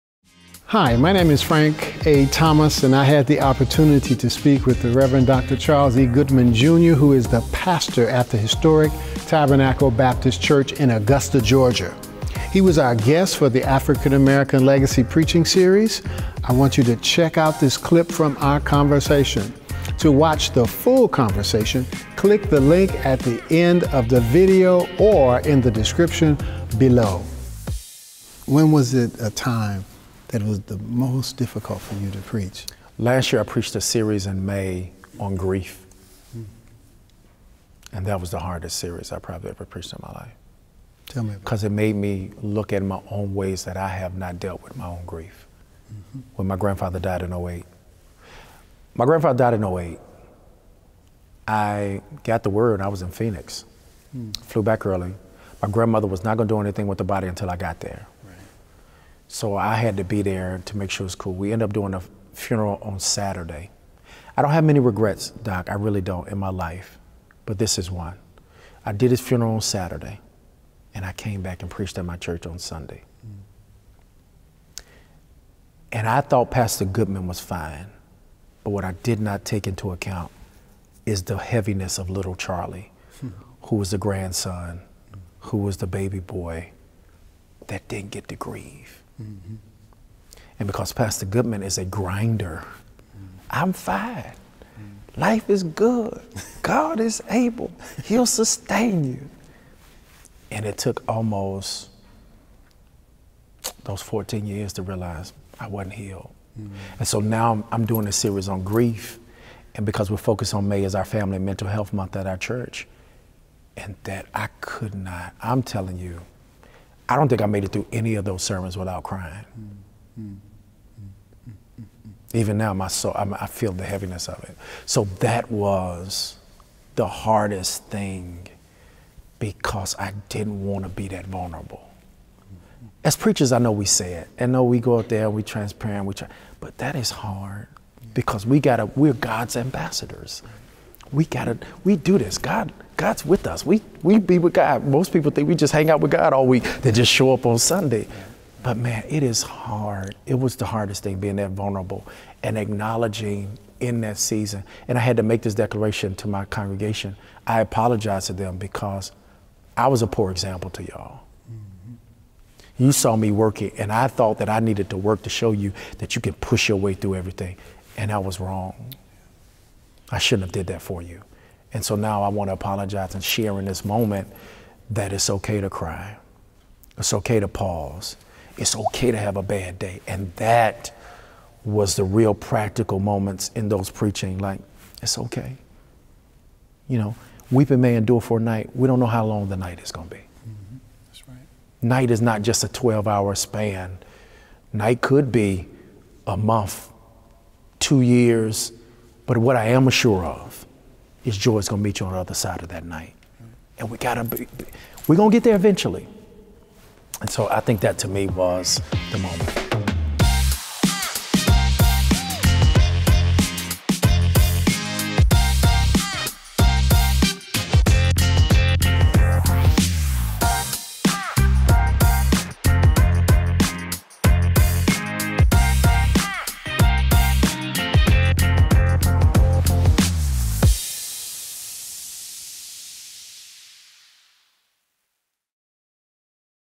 “The Preacher’s Grief” – Rev. Dr. Charles E. Goodman, Jr. – Legacy Preaching Series
A short clip from “A Conversation with Rev. Dr. Charles E. Goodman, Jr. hosted by Dr. Frank A. Thomas” for the African American Legacy Preaching Series.